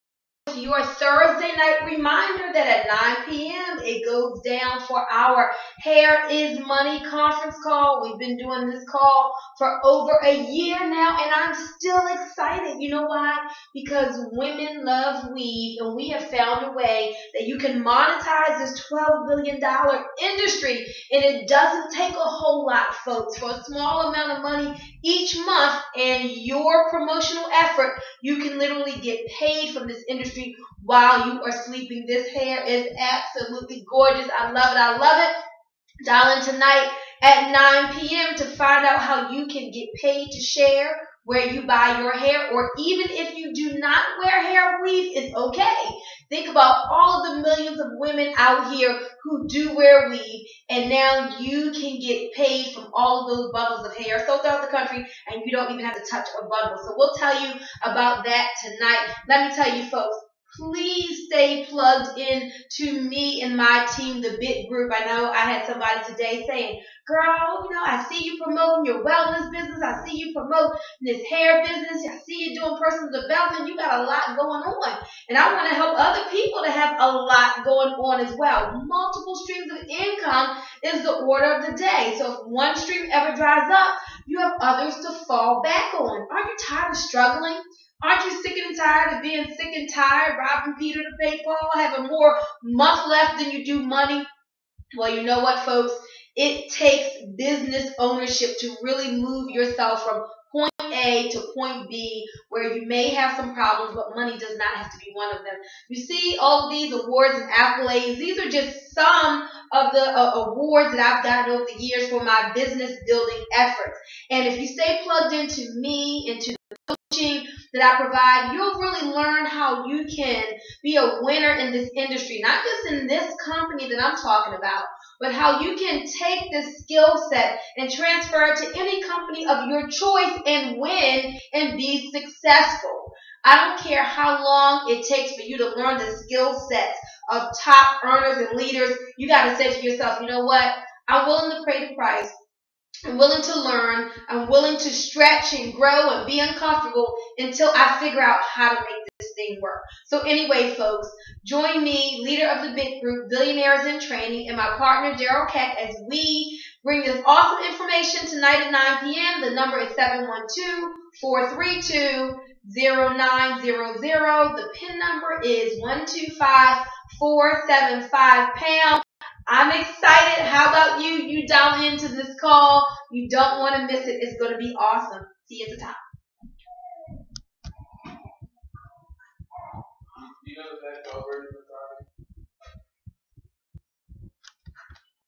Make money from your own online hair weave store
Every thursday at 9pm est. I lead an opportunity call for my team to explain how you can monetize the 12 billion dollar hair weave industry.